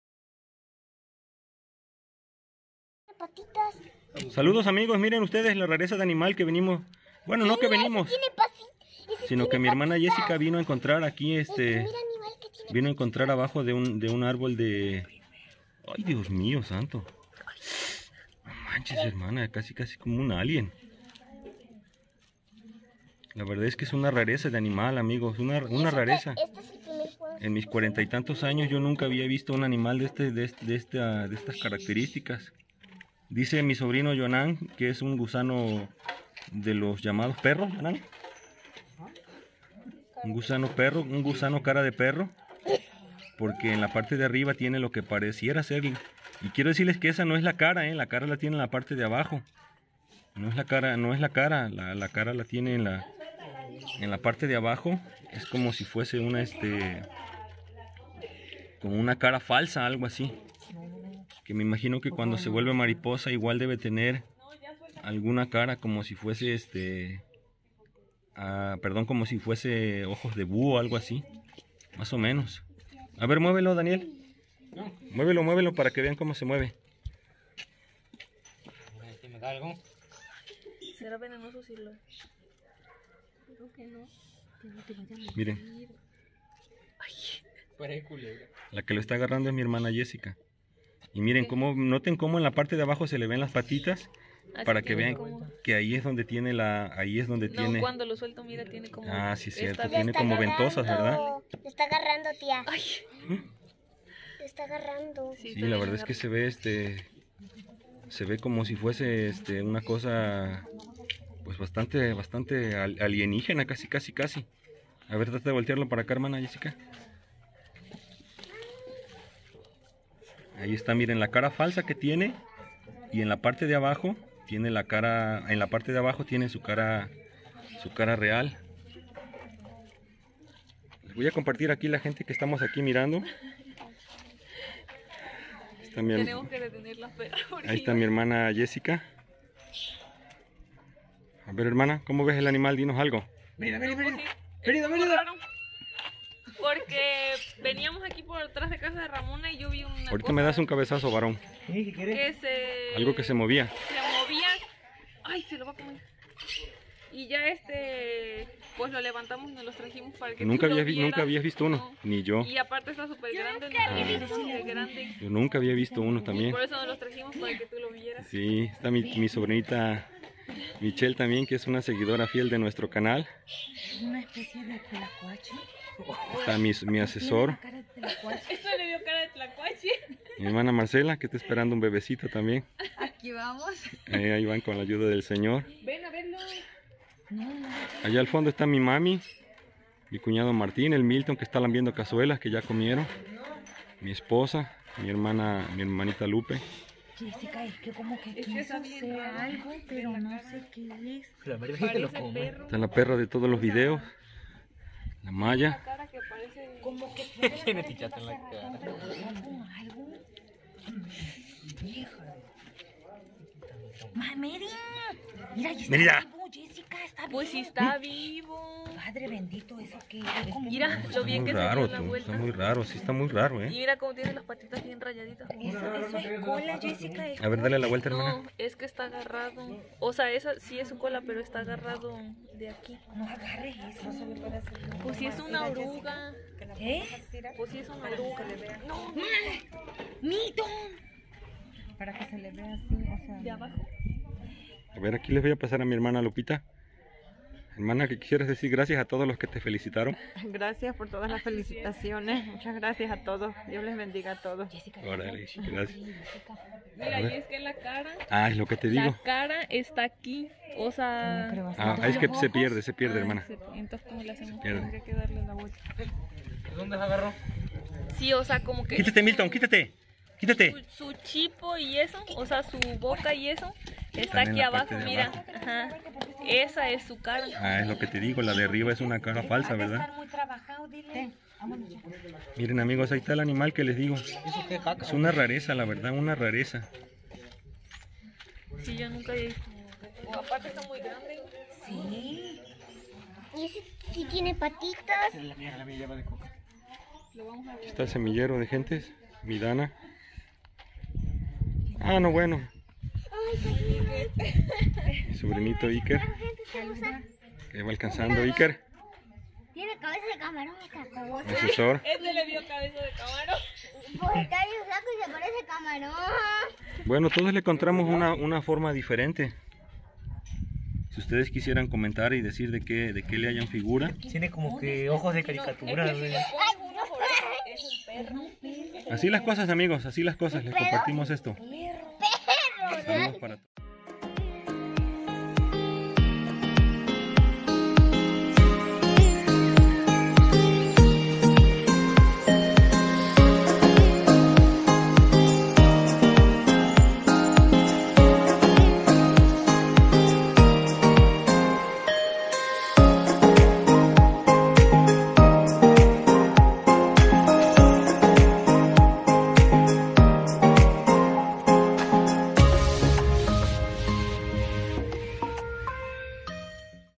Miren la creatura que encontró mi hermana Jessica
Este extraño y hermoso gusano, lo encontró mi hermana Jessica abajo de un tulipan de la India, quise documentarlo y dejarlo aquí, porque nunca en mis 44 años había visto algo semejante en mi comunidad Pinoltepec, no tenía idea que existiera